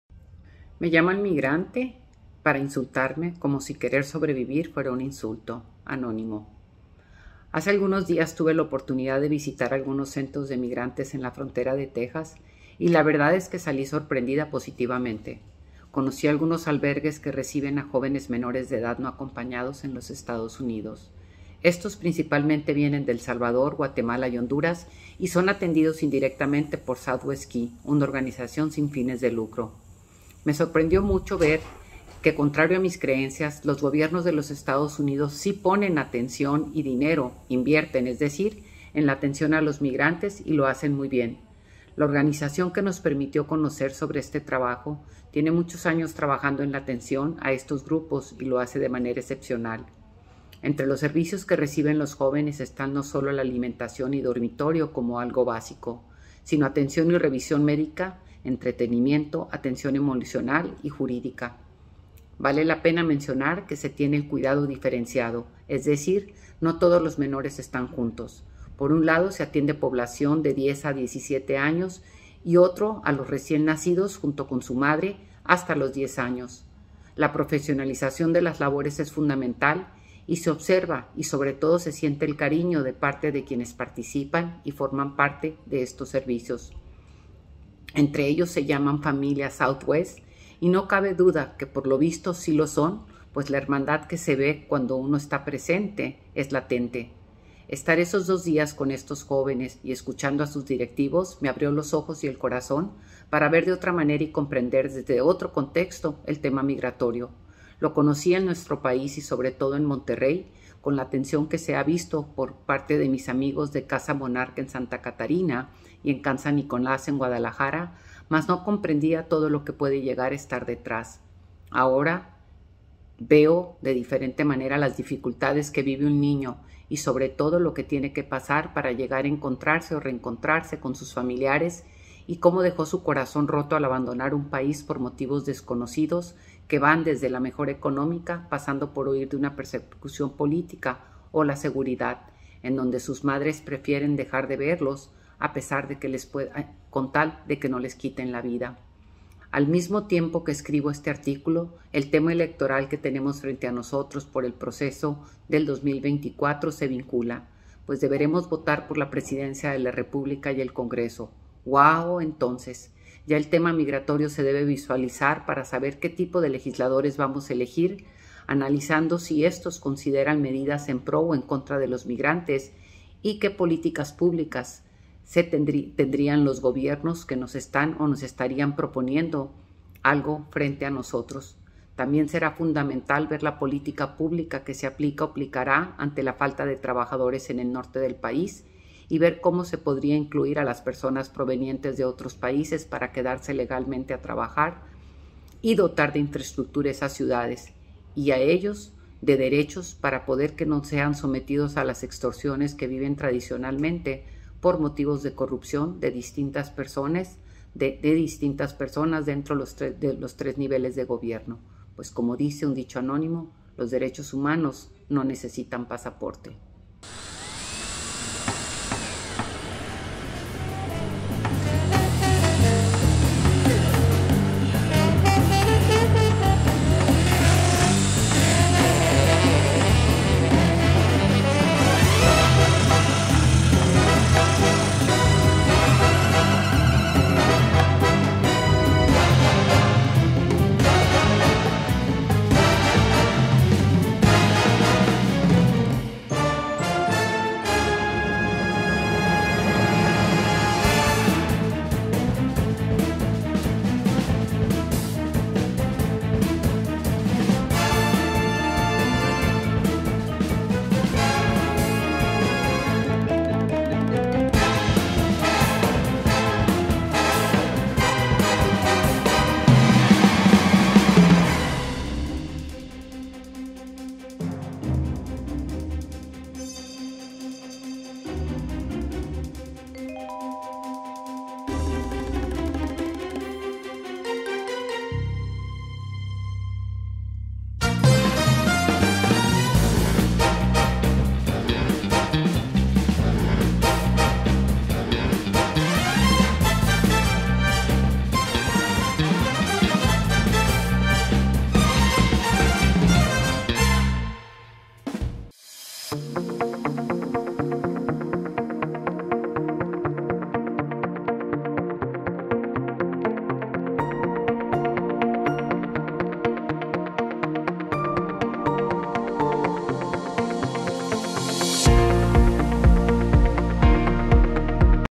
Me llaman inmigrante para insultarme. Por Tatiana Clouthier | Video columna
Wow entonces, ya el tema migratorio se debe visualizar para saber que tipo de legisladores vamos a elegir, analizando si estos consideran medidas en pro o contra los migrantes y que políticas públicas  que tendrían los gobiernos que nos están o estén proponiendo algo frente a nosotros.